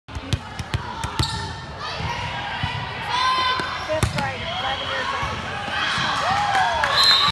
Age 11 Libero Jump serve against 15s
#5 Caleigh